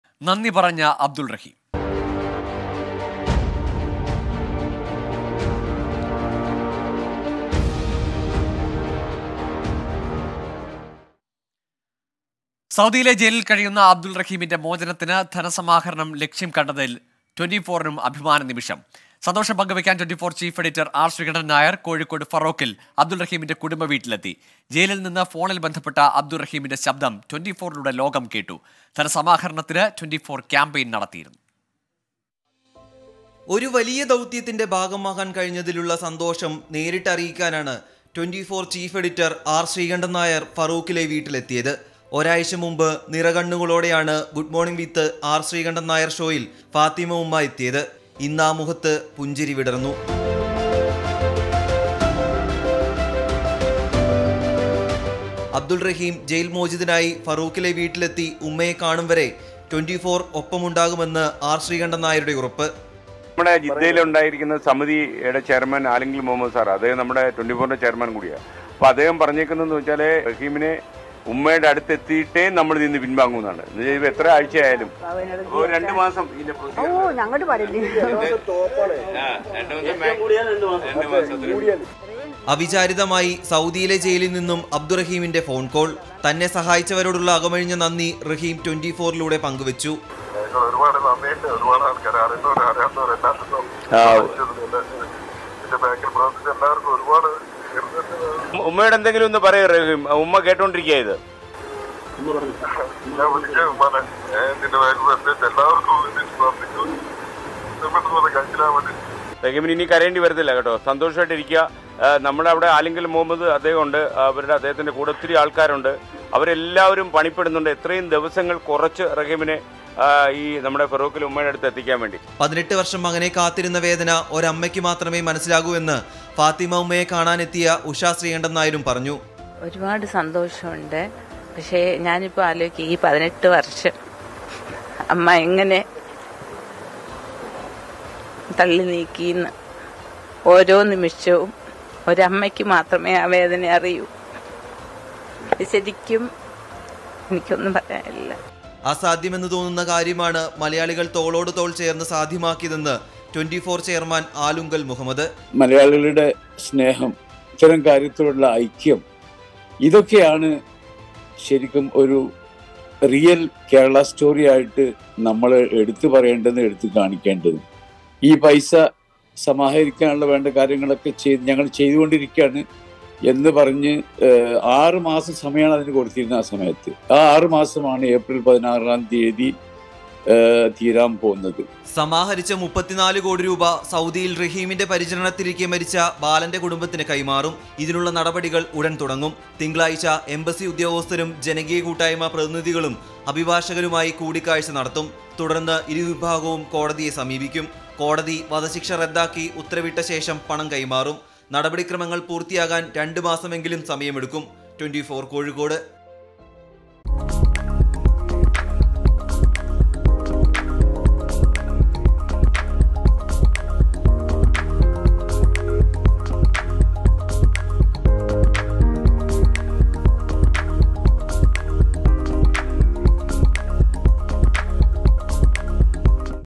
ധനസമാഹരണം ലക്ഷ്യം കണ്ടതിൽ ട്വന്റിഫോറിനും അഭിമാന നിമിഷം; അബ്ദുൽ റഹിമിന്റെ വീട്ടിലെത്തി SKN
ധനസമാഹരണം ലക്ഷ്യം കണ്ടതിൽ ട്വന്റിഫോറിനും അഭിമാന നിമിഷം; ഫാറോക്കിലെ അബ്ദുൽ റഹിമിന്റെ വീട്ടിലെത്തി എസ്കെഎൻ